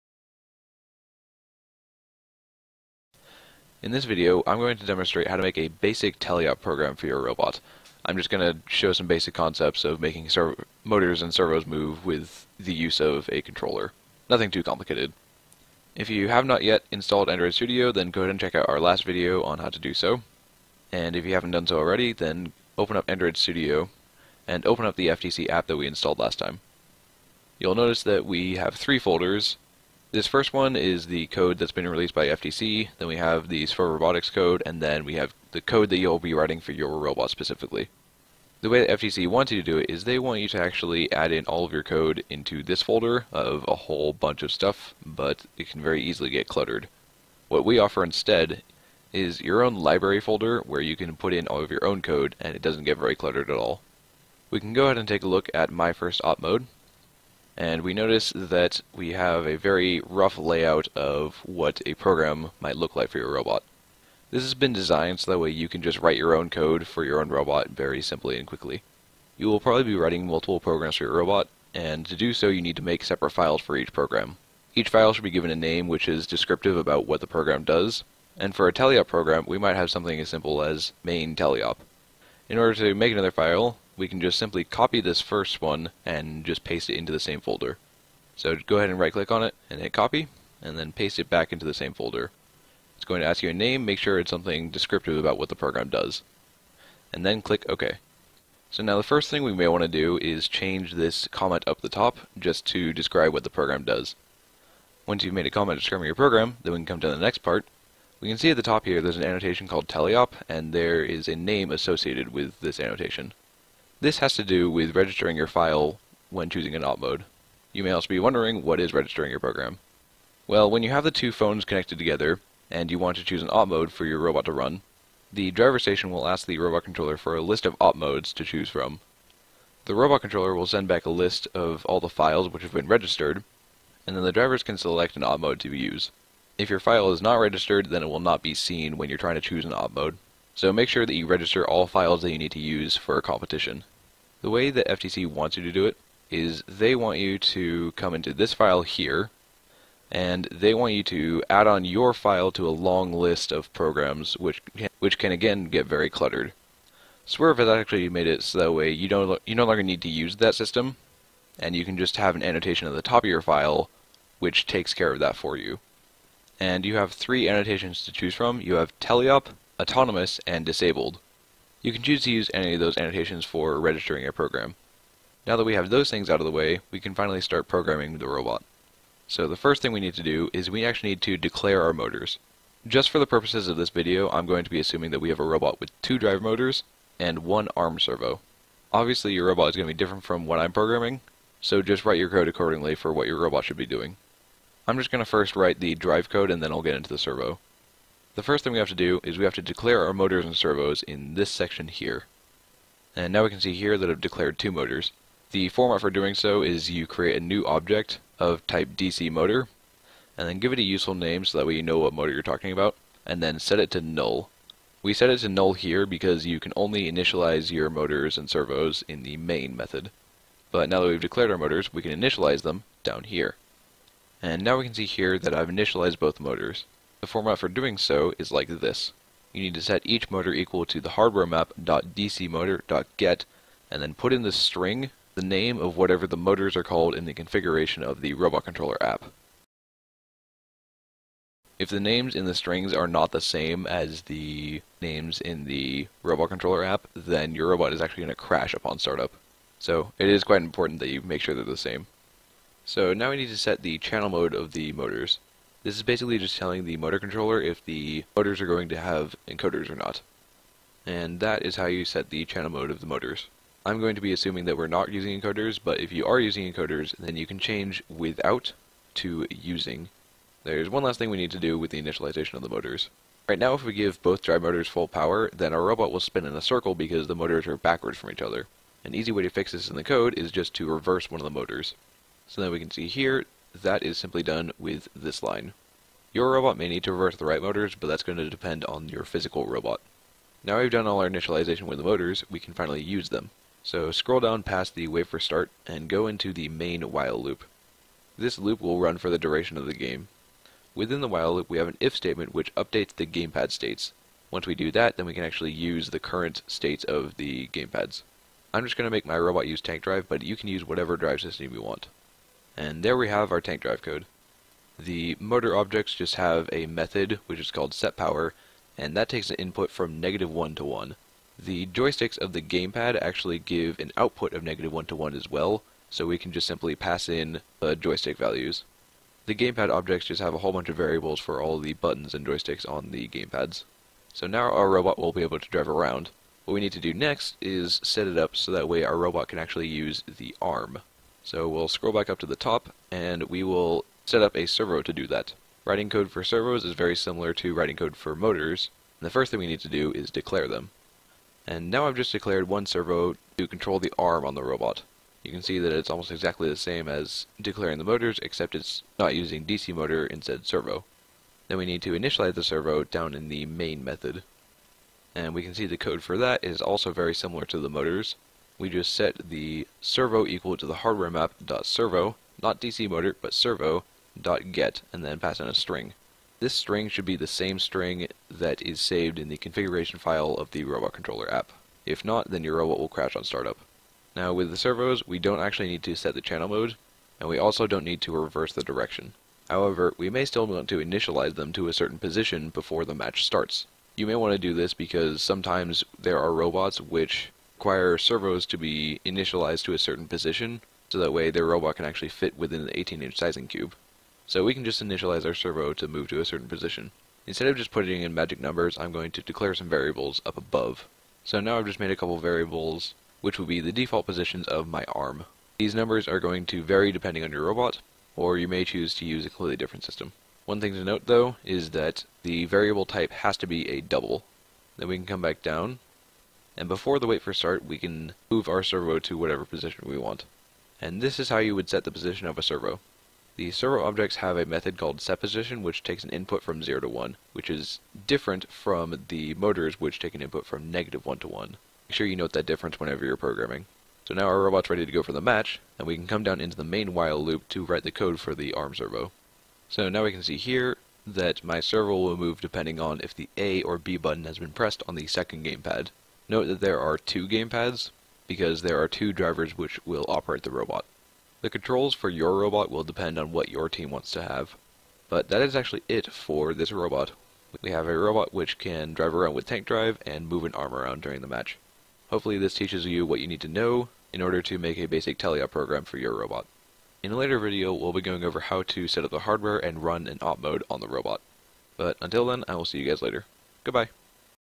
(Old) FTC Tutorials: Basic TeleOp Program
We go over the basics of programming motors and servos to move, as well as taking gamepad input.

All of our programming videos will use the Swerve Robotics code library, which is different from the FTC code library.